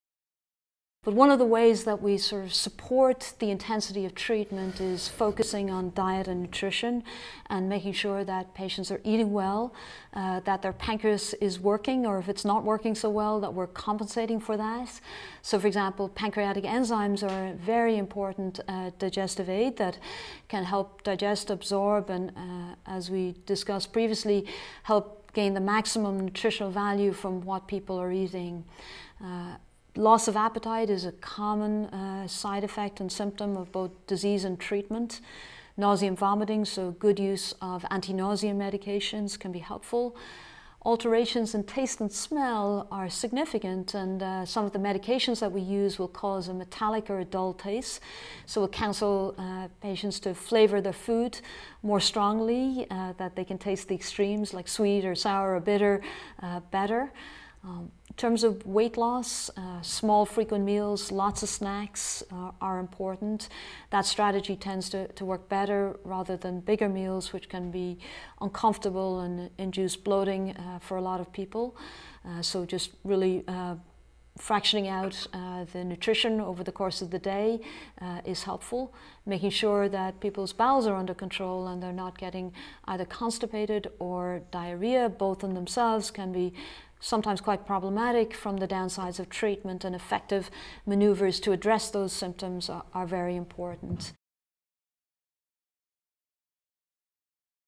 Managing Side Effects for Pancreatic Cancer Treatments
PancreaticCancer and its treatments can cause many different types of side effects. Learn tips and tricks for managing these side effects in this quick clip, and find more expert information in the full, free video education program